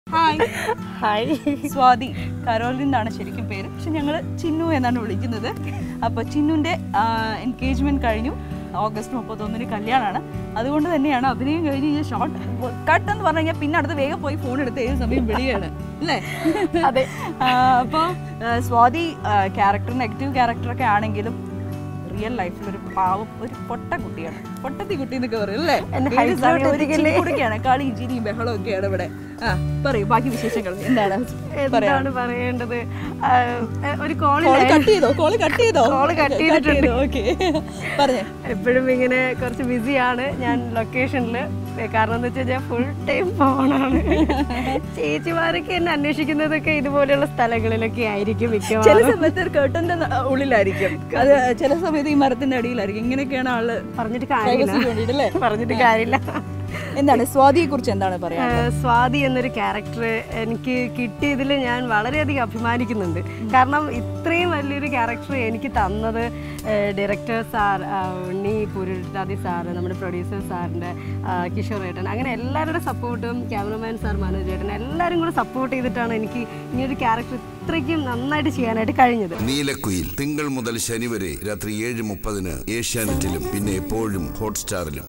നീലക്കുയിലിൽ പ്രേക്ഷക ഹൃദയം കവർന്ന വില്ലത്തി സ്വാതിയുടെ വിശേഷങ്ങൾ.
Neelakkuyil || Mon - Sat 7:30 PM || Asianet